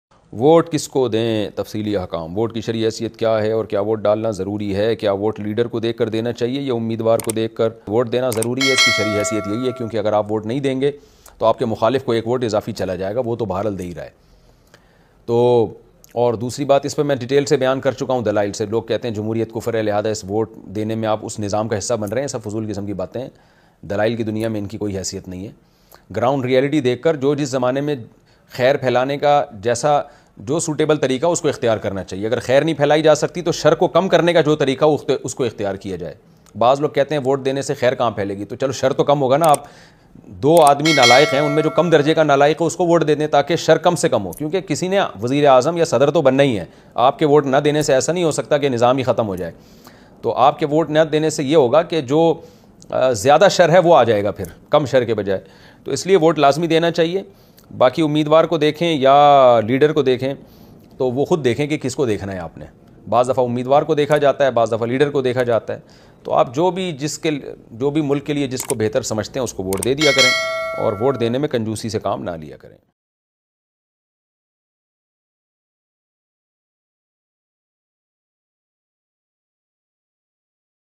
why is it important to vote By Mufti Tariq Masood#youtube
Vote Dena Kyu Zaroori Hai By Mufti Tariq Masood

Dena kyu zaroori hai islamic vote Dena kyu zaroori hai